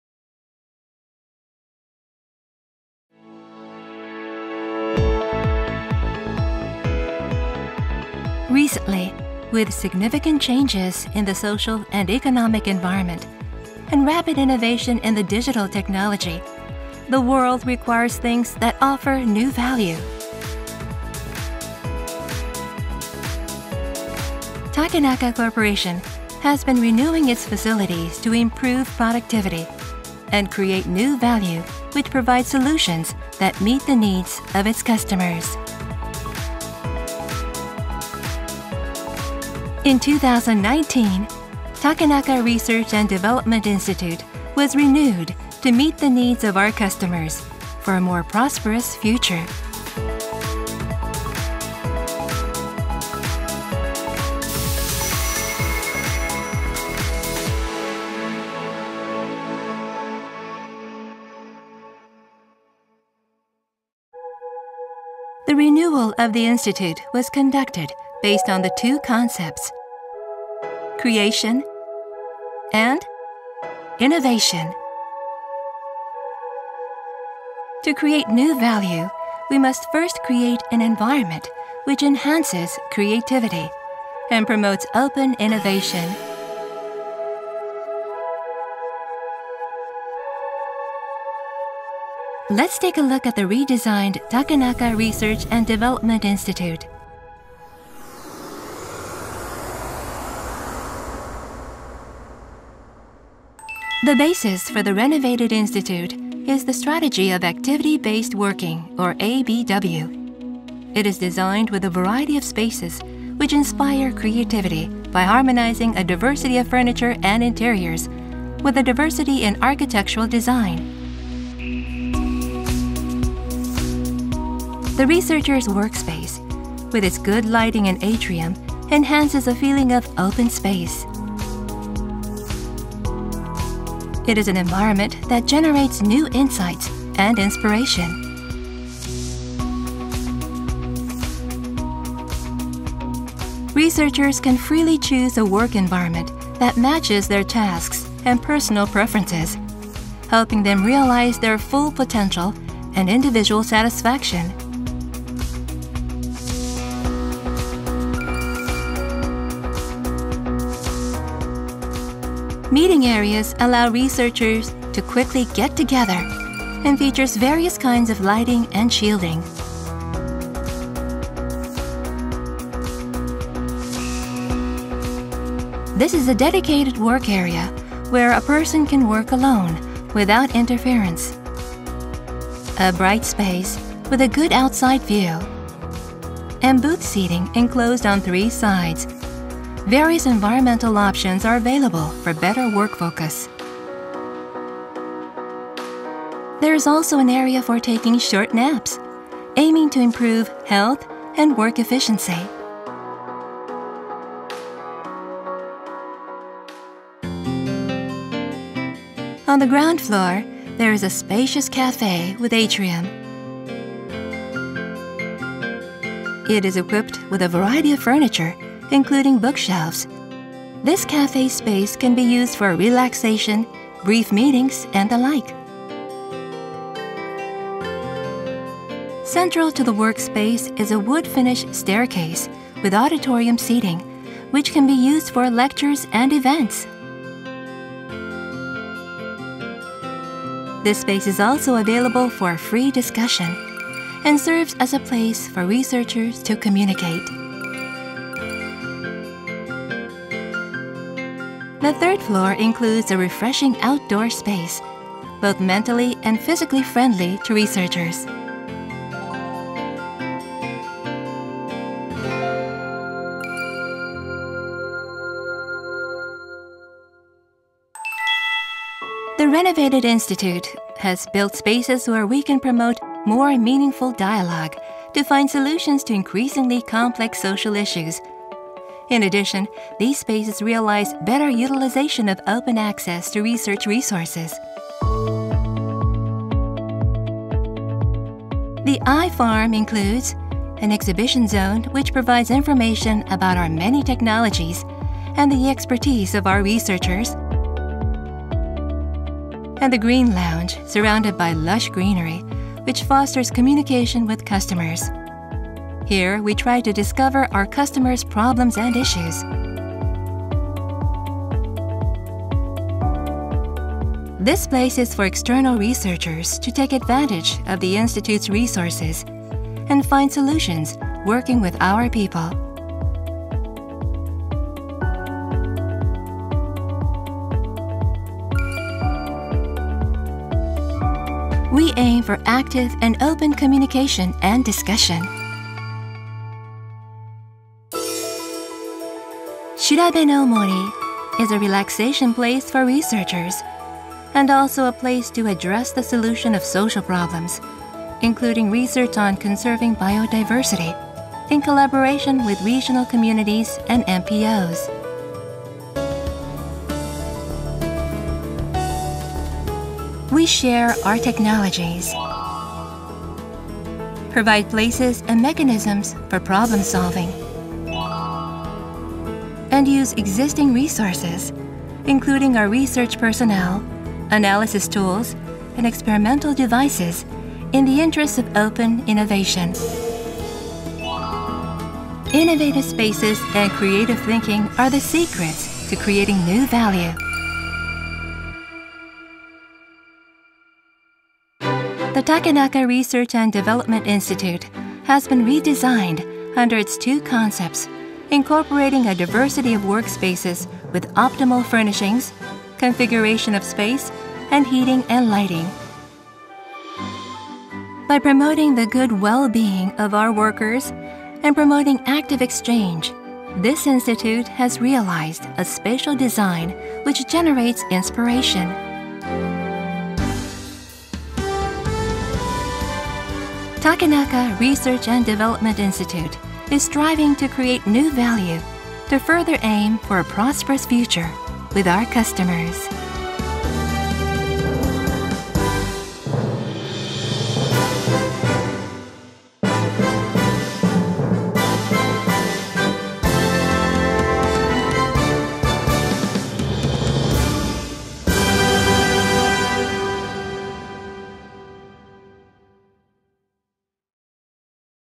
Takenaka Coporation The Creation of New Value－The Takenaka Research & Development Institute Renewal
Our institute was reborn to promote innovation through work styles and collaboration with partners.